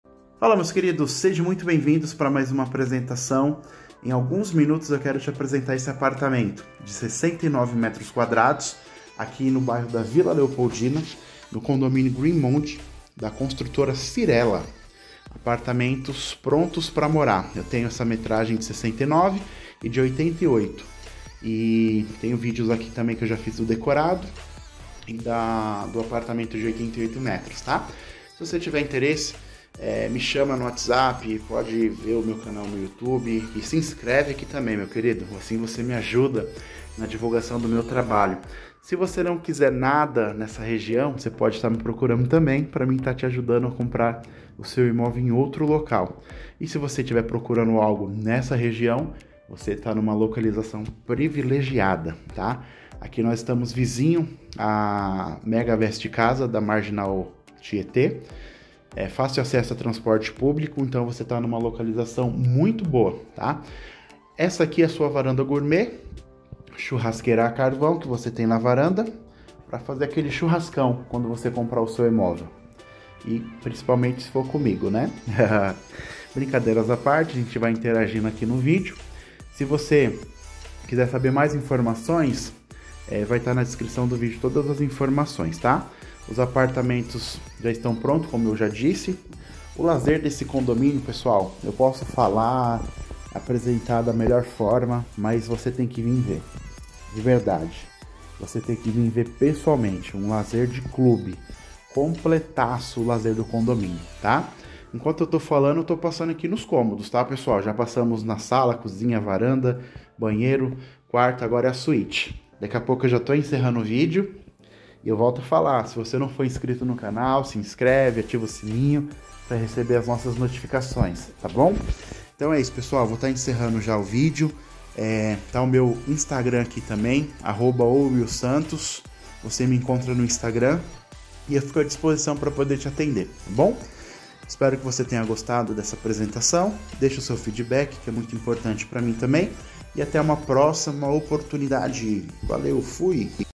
Apartamento venda, 69M2, com 2 dorm, 1 vaga. varanda goumert, green mond, vila leopoldina, São Paulo
Olá.

Sou Wil Santos corretor de imóveis responsável por esse imóvel.

CONDOMÍNIO CYRELA GREEN MOND
R. Lauriano Fernandes Júnior, 130 - Vila Leopoldina, São Paulo - SP, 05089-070

Seu Novo Lar na Vila Leopoldina - Conheça o Green Mond da Cyrela!

Descubra o equilíbrio perfeito entre conforto, modernidade e bem-estar no coração da Vila Leopoldina. Apresentamos o Green Mond, um empreendimento excepcional da renomada construtora Cyrela, pronto para receber você e sua família.

69m² de Espaço Elegante
Este amplo apartamento de 69m² oferece um layout inteligente, com dois quartos bem distribuídos e uma suíte luxuosa, garantindo privacidade e aconchego para todos os moradores. Os três banheiros garantem praticidade e conforto, atendendo a todas as necessidades do dia a dia.

Duas Vagas de Garagem
Não se preocupe com estacionamento. Com duas vagas de garagem, você e sua família terão total comodidade e segurança para guardar seus veículos.

Condomínio Maravilhoso com Área de Lazer Completa
O condomínio Green Mond oferece uma área de lazer completa que vai além das suas expectativas. Relaxe na piscina exuberante, mantenha-se ativo na academia moderna e compartilhe momentos especiais nos espaços de convivência. Seus filhos vão adorar o playground e a brinquedoteca, enquanto você desfruta de momentos relaxantes nos jardins bem cuidados.

Vista Excepcional e Localização Privilegiada
Desfrute de uma vista privilegiada da região da Vila Leopoldina, com suas ruas arborizadas e ambiente tranquilo. Além disso, a localização do Green Mond oferece fácil acesso a pontos-chave da cidade, como escolas, supermercados, centros comerciais e vias de transporte.

Invista no Seu Futuro Hoje
O Green Mond é mais do que um apartamento; é um investimento no seu estilo de vida e no seu futuro. Não perca a oportunidade de fazer parte deste empreendimento que reúne sofisticação, conforto e qualidade de vida.

Agende uma visita e venha conhecer pessoalmente todas as vantagens de viver no Green Mond. Entre em contato conosco para mais informações e garanta o seu espaço neste condomínio incrível.

Conheça melhor meu trabalho.